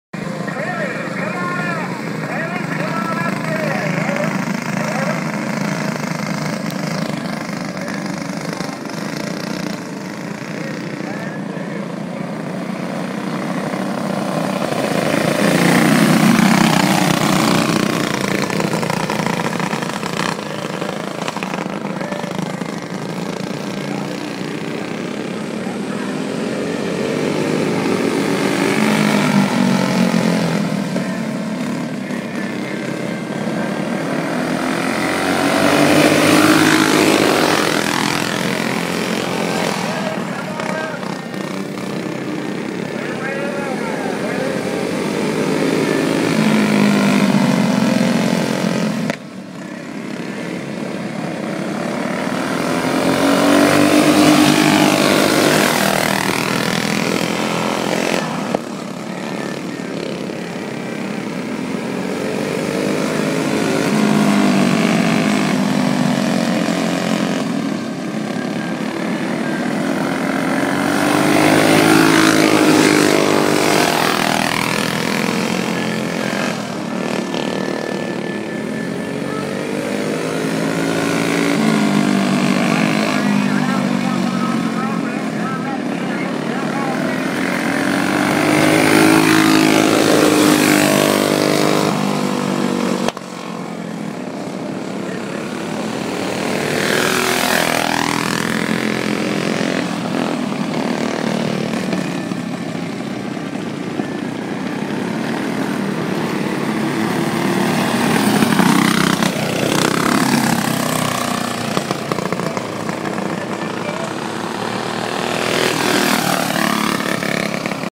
8-25-12 Hot Laps,CHAMPS, CHAMPIONS, CAGE CARTS,ANIMALS, GO CART RACING, HELTONVILLE INDIANA
This is a nice fun clean track! Filled with lots of competition and lots of different classes! North on 446 past 58 blinker lights on right at end of southbound truck lane! Look for small sign!